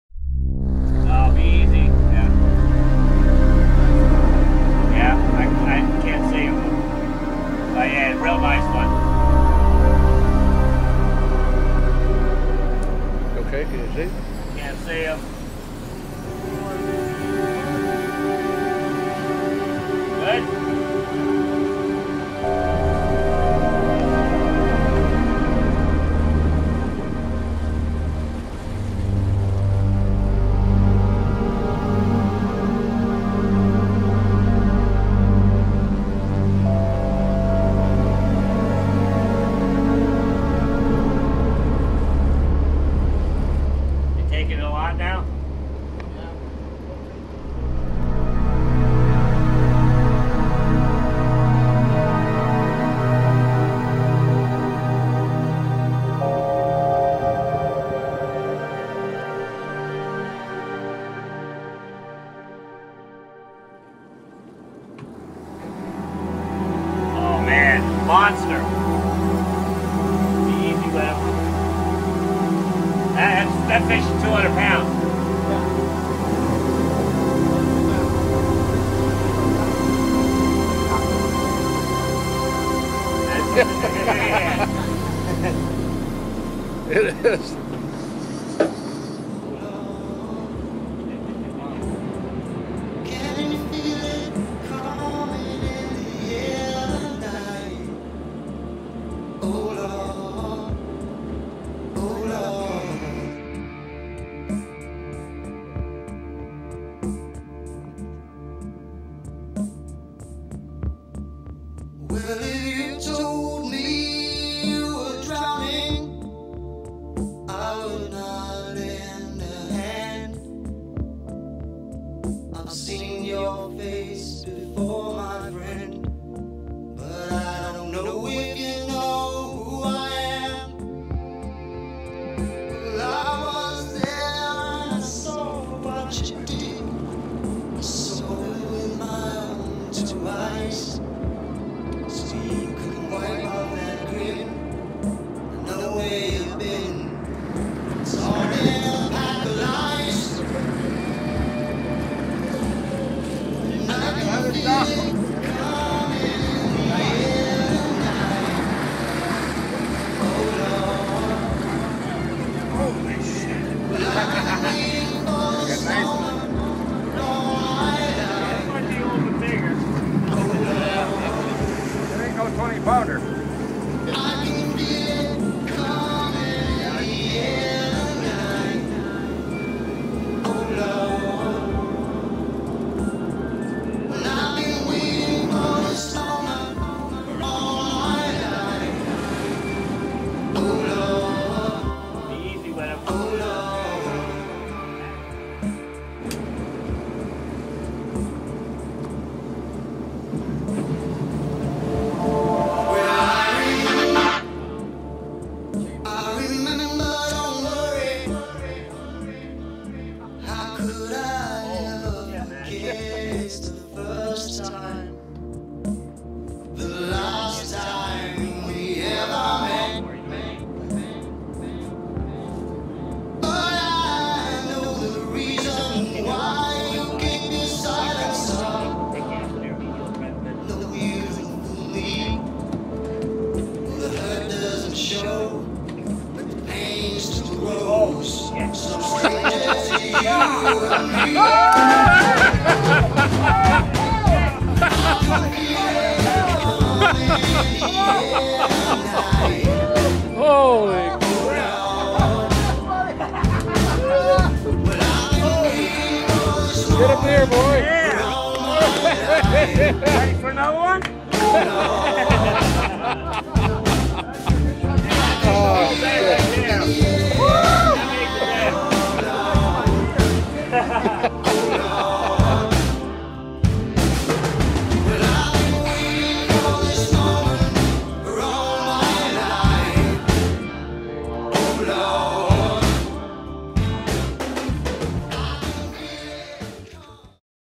250 Pound Yellowfin Tuna!!!
This was the catch of a lifetime for me in Los Suenos, Costa Rica.  It was an epic battle lasting 1 hour and 10 minutes.  We were aboard The Mako and the crew did an excellent job helping me bring in the monster 250 lb Yellowfin!